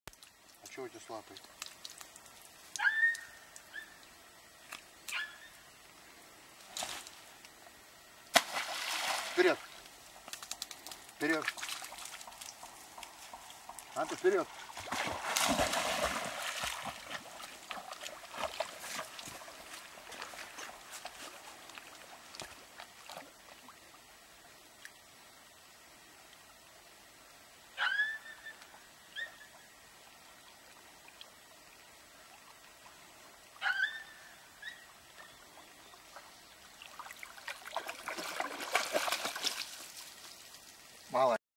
Тренировка подачи утки :: Ханта :: часть 2
работа карелок по утке